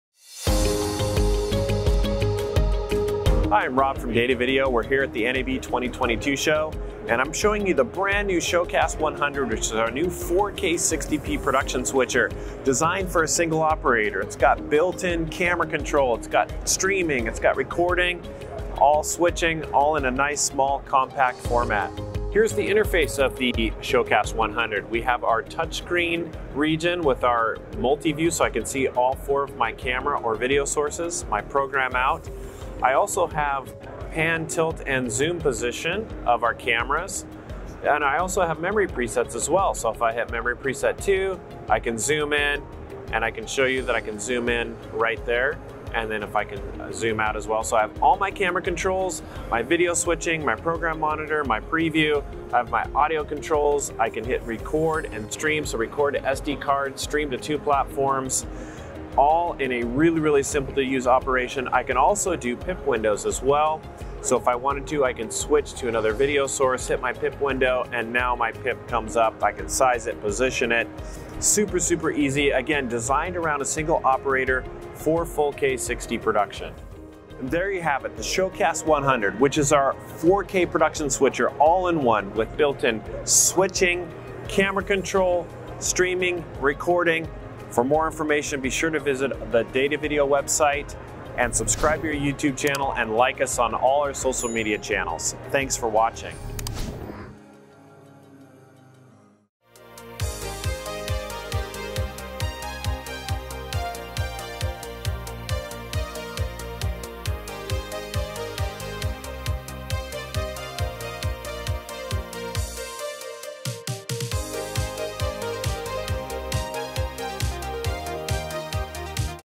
New at NAB 2022 | SHOWCAST 100 4K ALL-IN-ONE PRODUCTION STUDIO
The ShowCast 100 is a brand new 4K 4-Channel Touch Panel Switcher that features 7 major functions. It can switch video, overlay text and graphics, control cameras, live stream, record, and mix audio all from a single unit. Similar to a tablet, the 14-inch touch panel allows easy and intuitive access to switcher functions.

CAMERA CONTROL
The ShowCast 100's built-in camera control functions allow you to simultaneously control three PTZ cameras. You can directly save the lens position presets, pan and tilt camera lens in real time, zoom in and out of an image, and adjust the white balance, iris and mirror mode.

TOUCH SCREEN SWITCHER CONTROLS
Virtual buttons on the large touch screen panel makes it easy for operating the switcher as well as setting up presets for cameras. Operating the ShowCast 100 is as easy as using a tablet. Produce professional video productions without having a huge learning curve on how to use and operate a switcher.

VIDEO STREAMING AND RECORDING
The built-in dual live streaming encoder allows you to record and stream simultaneously. With the ShowCast 100's recording capability, you can easily back up the recorded video files for post production edits without much effort.